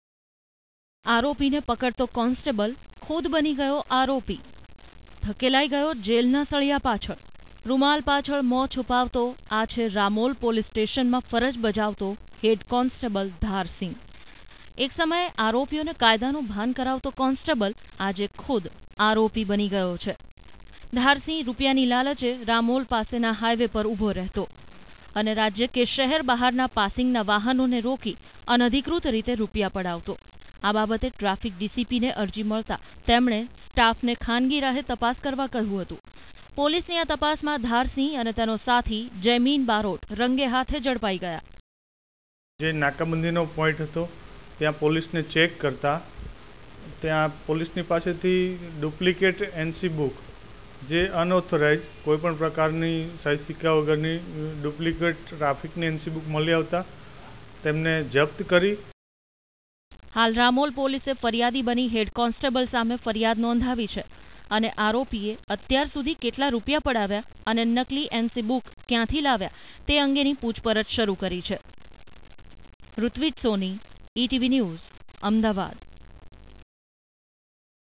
Ahmedabad: Ramol Police Station's Head constable caught in Robbing_Etv News Gujarati
Ahmedabad: Ramol Police Station's Head constable caught in
Robbing